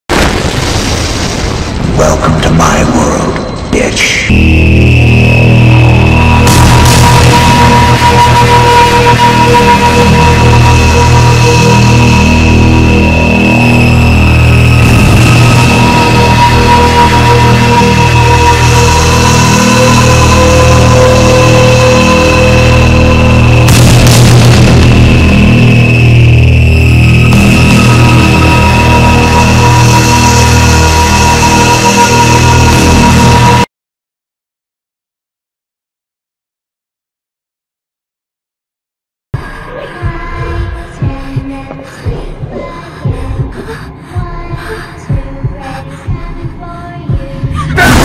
00 Freddy Krueger top (antagonist movie horror)
the attacks in your dreams and never woke up, freddy krueger is in the position 00 of the icons of horror
00 freddy krueger